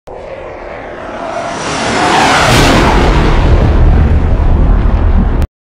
When a steam locomotive breaks the sound barrier going mach 1.1
mallard is nothing